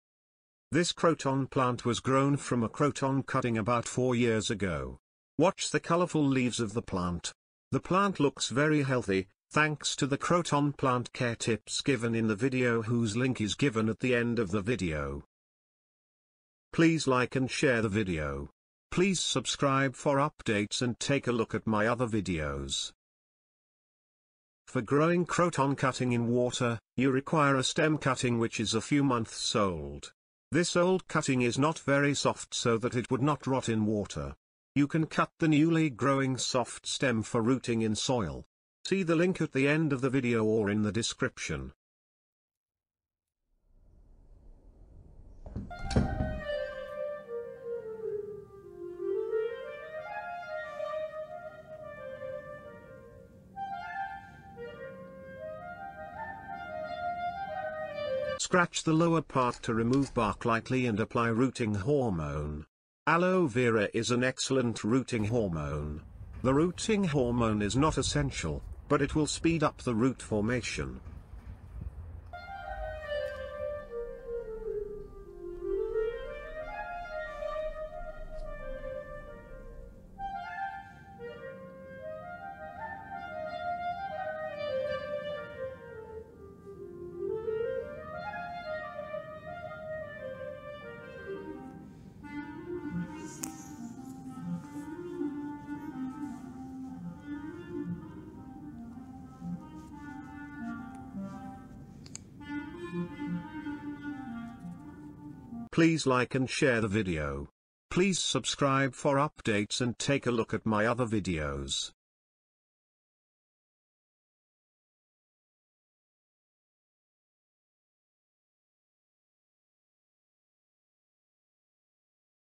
Croton Propagation In Water: How to Grow Croton Plant In Water
Learn the procedure for croton plant propagation in water from cuttings. Can you propagate croton in water, if yes, then how do you grow croton plant from cuttings in water?  The video shows step by step instructions on how to root croton cuttings in water. The articles and videos on croton plant growing and care tips are given below.